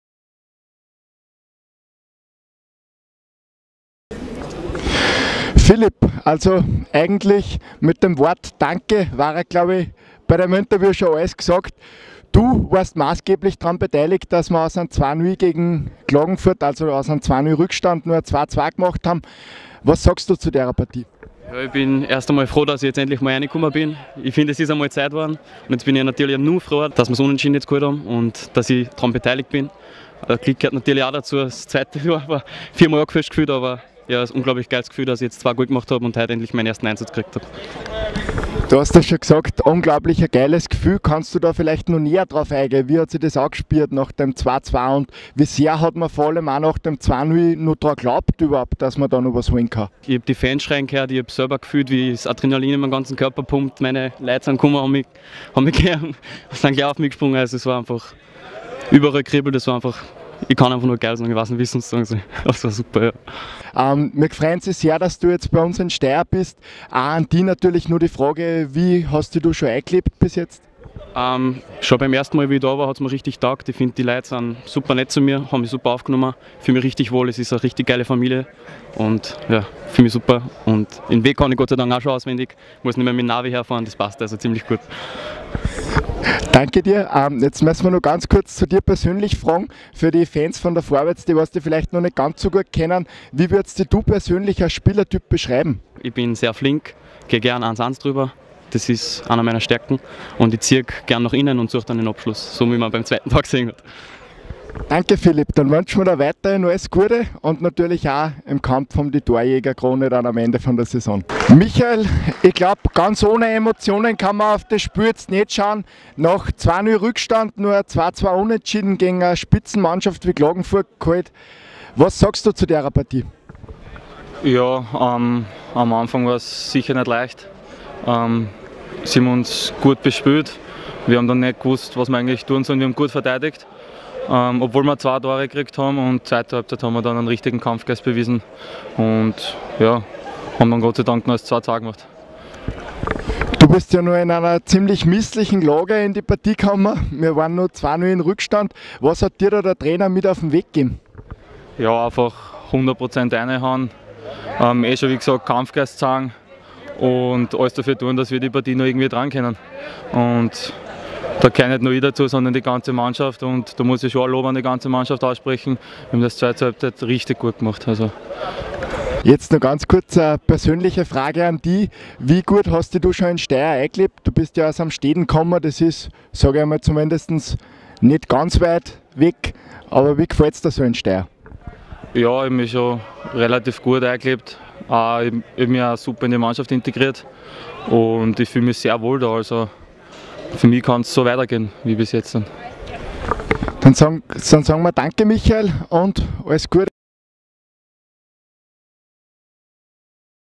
2. Liga, 4. Runde | Vorwärts Steyr vs. Austria Klagenfurt 2:2
In seinem ersten Spiel für den SKV erzielte Philipp Ablinger 2 Tore und bringt der Vorwärts den Ausgleich in der Nachspielzeit. Mit Michael Drga haben wir einen weiteren Neuzugang vor die Kamera geholt.